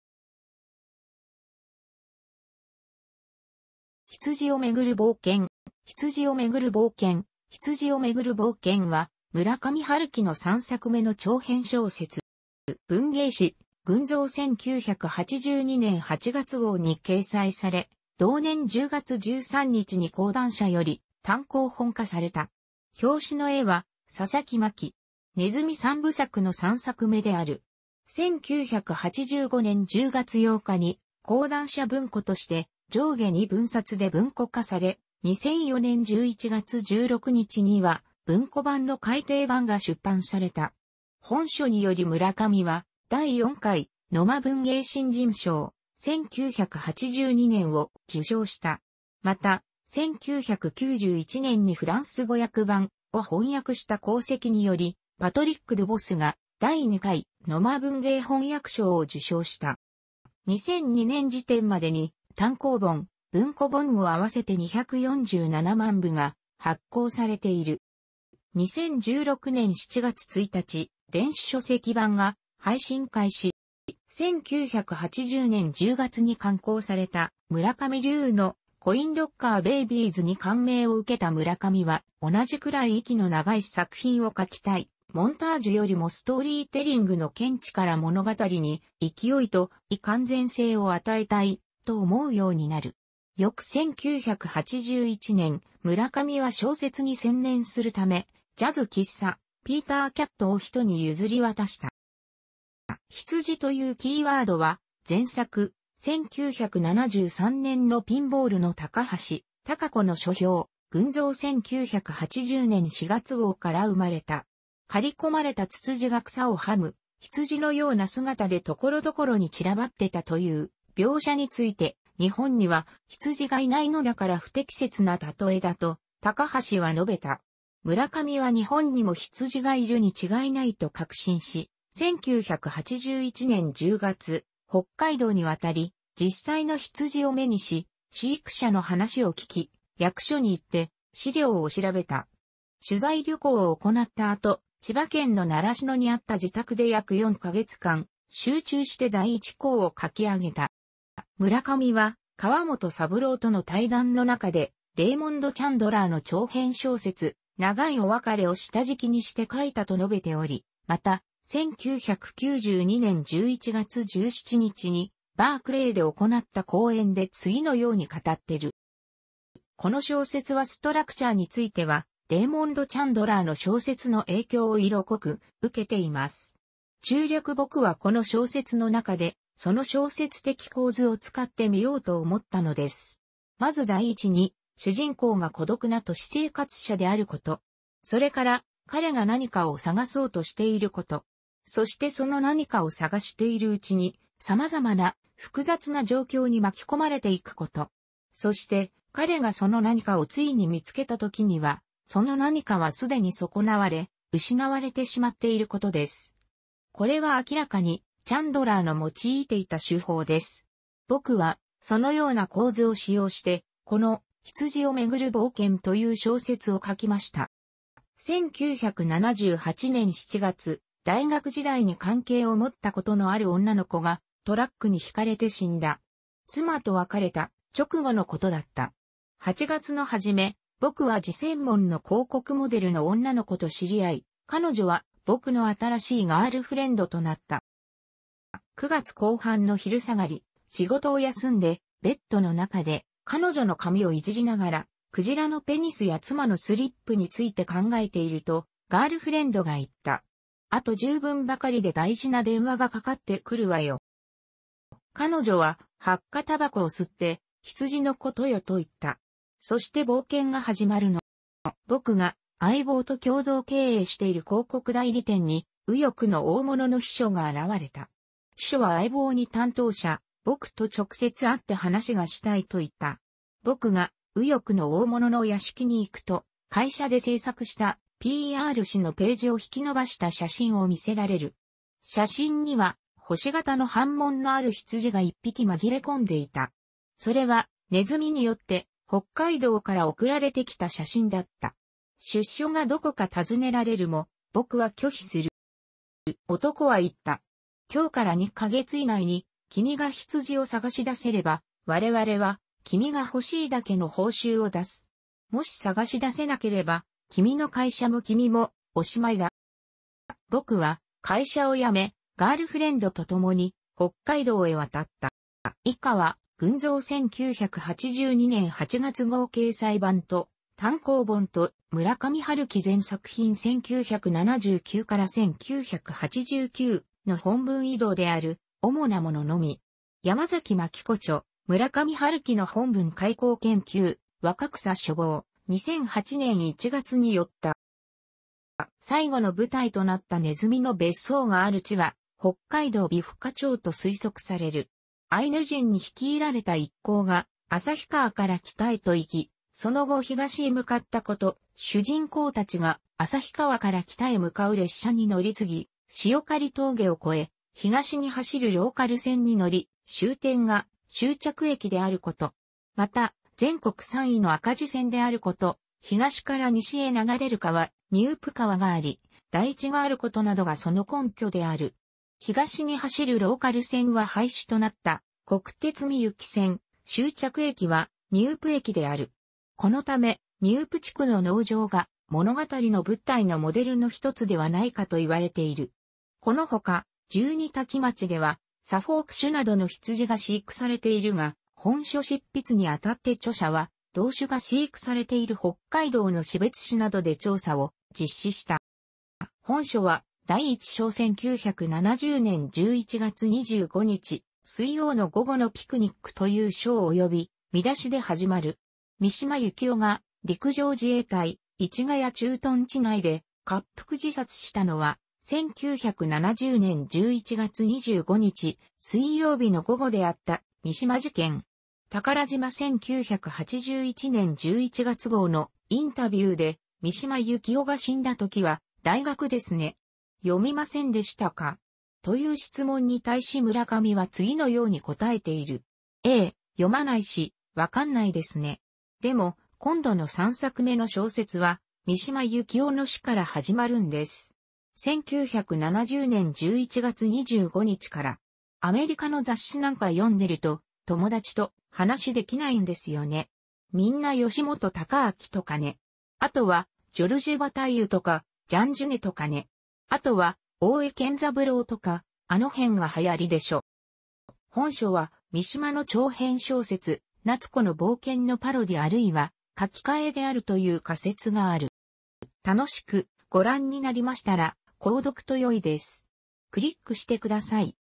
羊をめぐる冒険
『羊をめぐる冒険』（ひつじをめぐるぼうけん）は、村上春樹の3作目の長編小説。

文芸誌『群像』1982年8月号に掲載され、同年10月13日に講談社より単行本化された。表紙の絵は佐々木マキ。「鼠三部作」の3作目である。1985年10月8日に講談社文庫として上下二分冊で文庫化され、2004年11月16日には文庫版の改訂版が出版された。本書により村上は第4回野間文芸新人賞（1982年）を受賞した。また、1991年にフランス語訳版『』を翻訳した功績により、パトリック・ドゥヴォスが第2回野間文芸翻訳賞を受賞した。

2002年時点までに、単行本・文庫本を合わせて247万部が発行されている。

2016年7月1日、電子書籍版が配信開始。

1980年10月に刊行された村上龍の『コインロッカー・ベイビーズ』に感銘を受けた村上は、同じくらい息の長い作品を書きたい、モンタージュよりもストーリーテリングの見地から物語に勢いと強い完全性を与えたい、と思うようになる。翌1981年、村上は小説に専念するためジャズ喫茶「ピーター・キャット」を人に譲り渡した。

「羊」というキーワードは前作『1973年のピンボール』の高橋たか子の書評（『群像』1980年4月号）から生まれた。「刈りこまれたつつじが草をはむ羊のような姿でところどころにちらばっていた」という描写について、日本には羊がいないのだから不適切な喩えだと高橋は述べた。村上は日本にも羊がいるに違いないと確信し、1981年10月、北海道に渡り、実際の羊を目にし、飼育者の話を聞き、役所に行って資料を調べた。取材旅行を行った後、千葉県の習志野にあった自宅で約4か月間集中して第一稿を書き上げた。

村上は川本三郎との対談の中でレイモンド・チャンドラーの長編小説『長いお別れ』を下敷きにして書いたと述べており、また、1992年11月17日にバークレーで行った講演で次のように語っている。

「この小説はストラクチャーについてはレイモンド・チャンドラーの小説の影響を色濃く受けています。（中略） 僕はこの小説の中で、その小説的構図を使ってみようと思ったのです。まず第一に主人公が孤独な都市生活者であること。それから、彼が何かを捜そうとしていること。そしてその何かを捜しているうちに、様々な複雑な状況に巻き込まれていくこと。そして彼がその何かをついに見つけたときには、その何かは既に損なわれ、失われてしまっていることです。これは明らかにチャンドラーの用いていた手法です。僕はそのような構図を使用して、この『羊をめぐる冒険』という小説を書きました」

1978年7月、大学時代に関係を持ったことのある女の子がトラックに轢かれて死んだ。妻と別れた直後のことだった。8月のはじめ、「僕」は耳専門の広告モデルの女の子と知り合い、彼女は「僕」の新しいガール・フレンドとなった。

9月後半の昼下がり、仕事を休んでベッドの中で彼女の髪をいじりながら鯨のペニスや妻のスリップについて考えていると、ガール・フレンドが言った。「あと十分ばかりで大事な電話がかかってくるわよ」

彼女ははっか煙草を吸って「羊のことよ」と言った。「そして冒険が始まるの」

「僕」が相棒と共同経営している広告代理店に、右翼の大物の秘書が現われた。秘書は相棒に担当者（僕）と直接会って話がしたいと言った。「僕」が右翼の大物の屋敷に行くと、会社で製作したPR誌のページを引きのばした写真を見せられる。写真には星形の斑紋のある羊が一匹まぎれこんでいた。それは「鼠」によって北海道から送られてきた写真だった。出所がどこか尋ねられるも「僕」は拒否する。

男は言った。「今日から二ヵ月以内に君が羊を探し出せれば、我々は君が欲しいだけの報酬を出す。もし探し出せなければ、君の会社も君もおしまいだ」

「僕」は会社を辞め、ガール・フレンドと共に北海道へ渡った。

以下は『群像』1982年8月号掲載版と単行本と『村上春樹全作品1979～1989』の本文異同である（主なもののみ）。山﨑眞紀子著『村上春樹の本文改稿研究』（若草書房、2008年1月）に拠った。

最後の舞台となった鼠の別荘がある地は、北海道美深町と推測される。「アイヌ人に率いられた一行が、旭川から北へと行き、その後東へ向かったこと」、「主人公たちが、旭川から北へ向かう列車に乗りつぎ、塩狩峠を越え、東に走るローカル線に乗り、終点が終着駅であること。また全国三位の赤字線であること」、「東から西へ流れ...